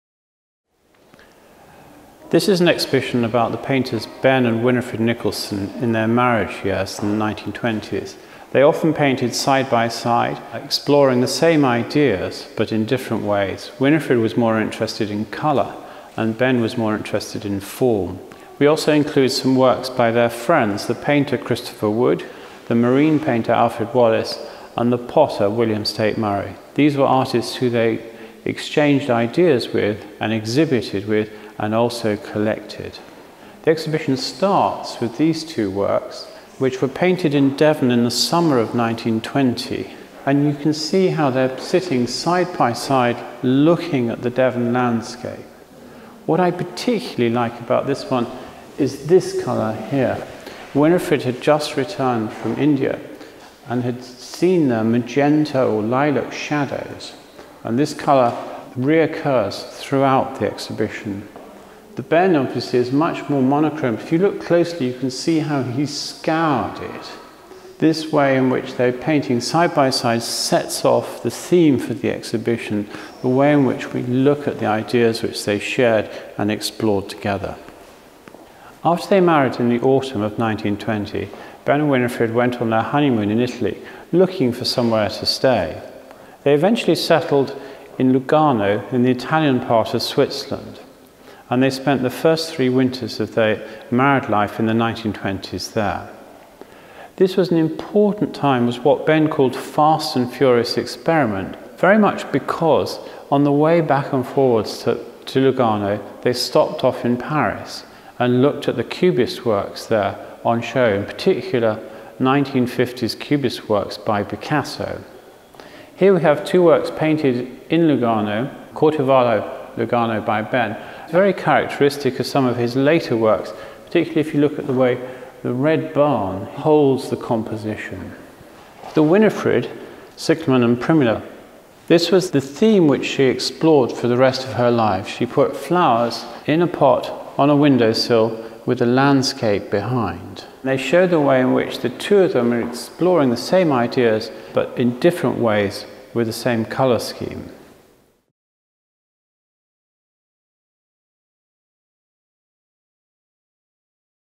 Dulwich Picture Gallery 'Art and Life' Curator's Tour: The Beginnings
Art and Life: Ben Nicholson, Winifred Nicholson, Christopher Wood, Alfred Wallis, William Staite Murray, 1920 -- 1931

Ben and Winifred Nicholson were at the forefront of the Modern British movement and produced some of the most memorable works of the period. This exhibition, curated by their grandson, Jovan Nicholson, provides a rare opportunity to see their views of the same landscapes, seascapes, still lifes and portraits alongside pieces by their contemporaries Christopher Wood, Alfred Wallis and the potter William Staite Murray.

Spanning over a decade of exciting artistic exchange, this is the first exhibition to consider the group of artists in the same context, over a period that shaped the careers of Ben and Winifred Nicholson irrevocably.

This exhibition continuing the Modern British series at Dulwich Picture Gallery is a collaboration between Dulwich Picture Gallery, Leeds Museums and Galleries (18 October 2013- 12 January 2014) and Kettle's Yard, University of Cambridge (15 February-11 May 2014).